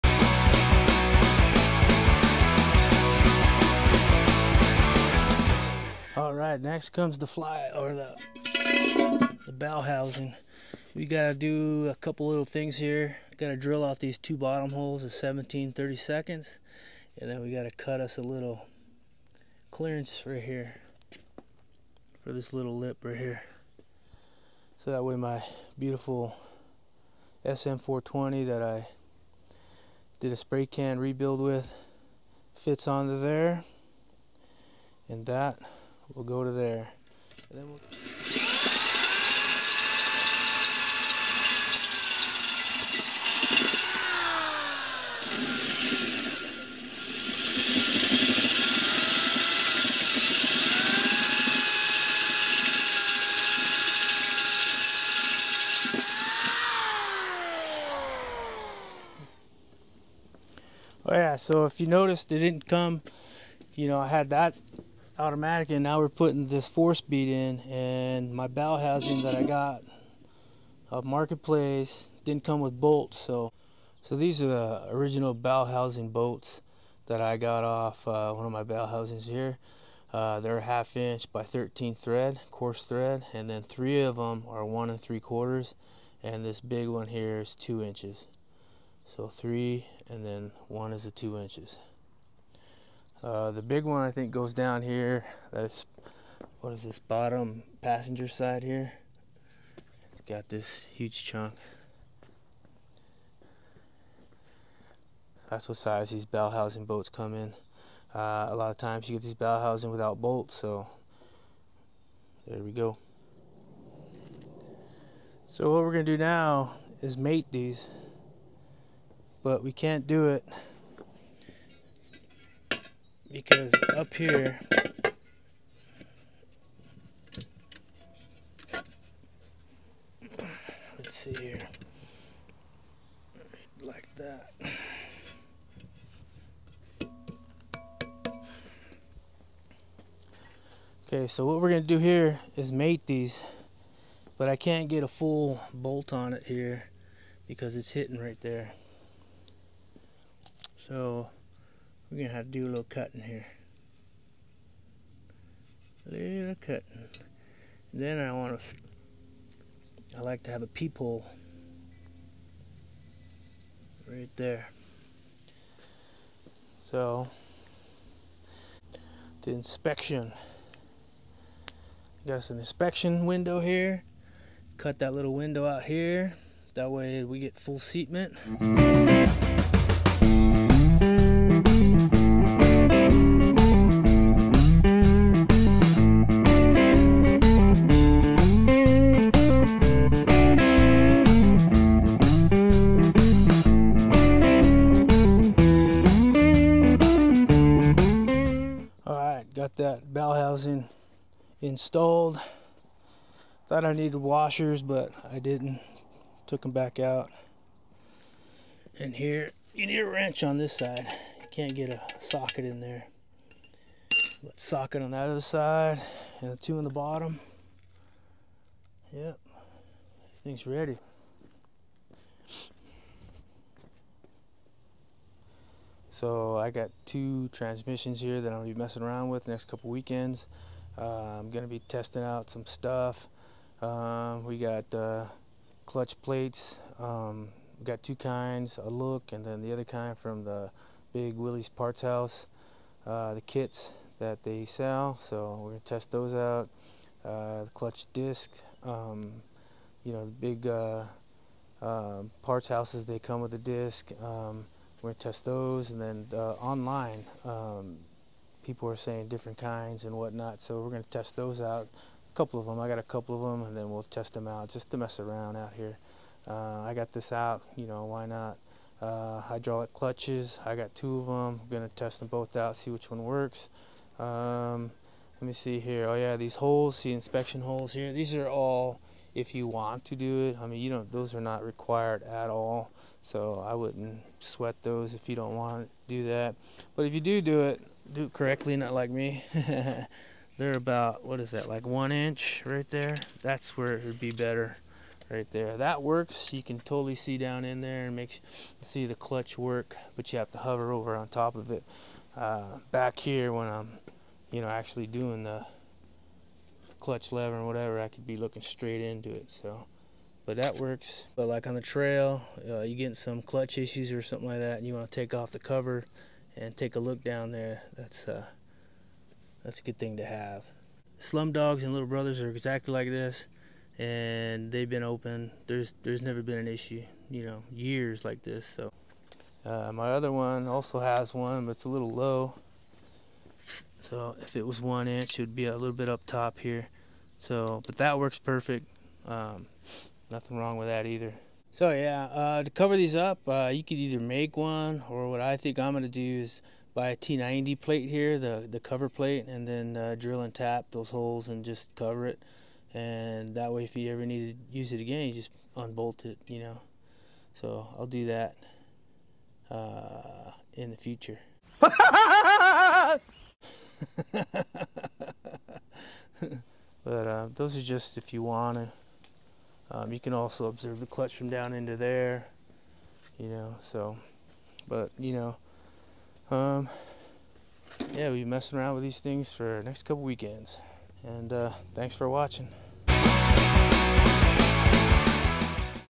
4 speed swap part 2/ setting up bellhousing to Buick 225 V6/ SM420/ CJ2A/Willys Jeep/ Flat Fender
Video my progress on my 4 speed swap sm420 to Buick v6 225 using Novak adapter, Novak adjustable throw out bearing,  and Novak clutch fork.
And just having fun with my little jeep in the back yard.